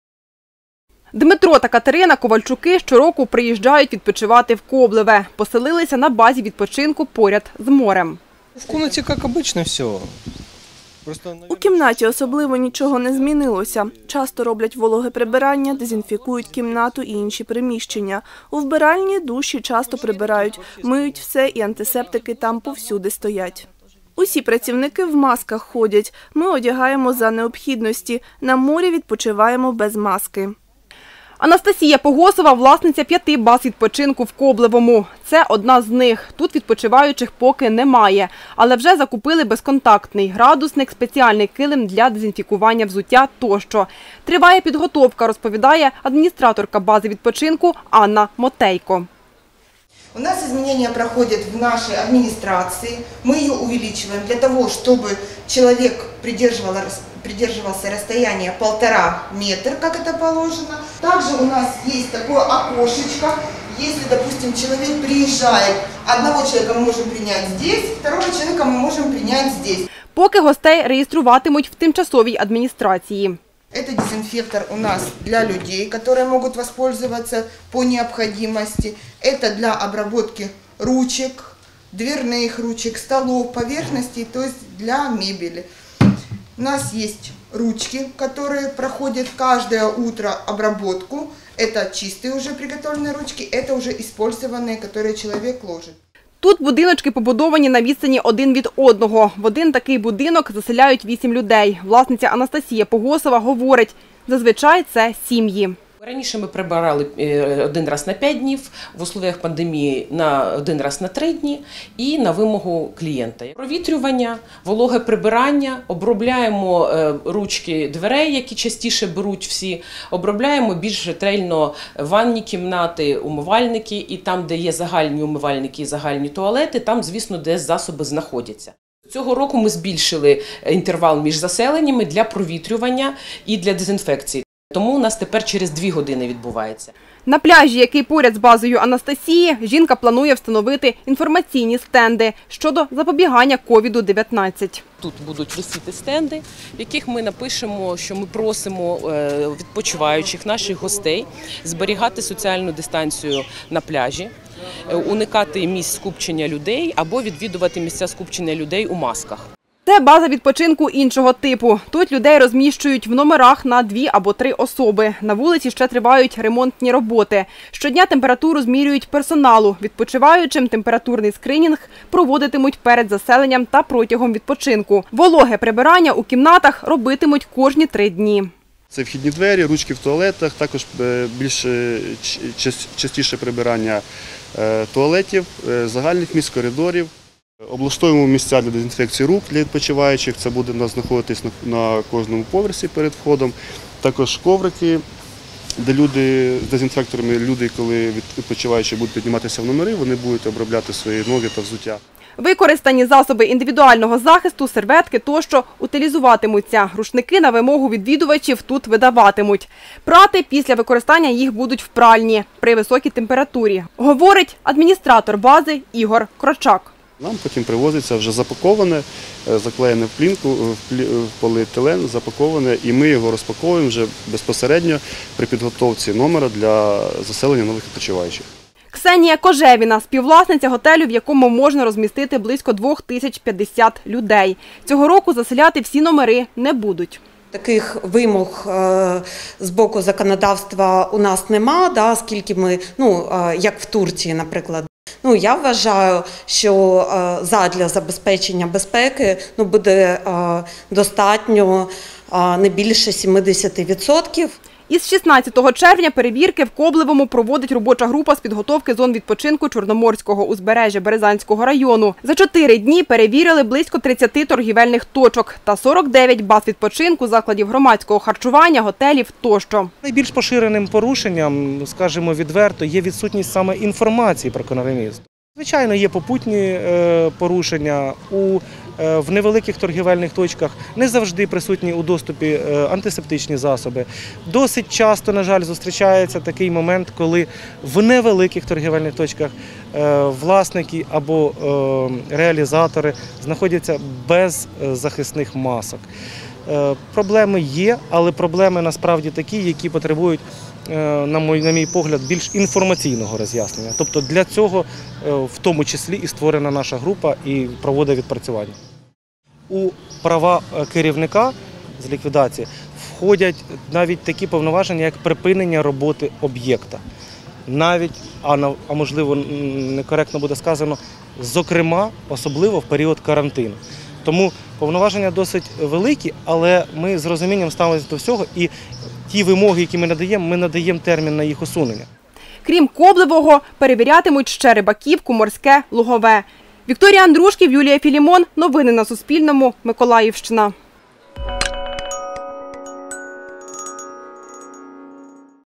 У Коблевому розпочався туристичний сезон
У Миколаївській області розпочався туристичний сезон. В курортній частині села Коблеве вже працюють готелі, заклади харчування, бази відпочинку тощо.
Авторка – Вікторія Андрушків, операторка – Юлія Філімон.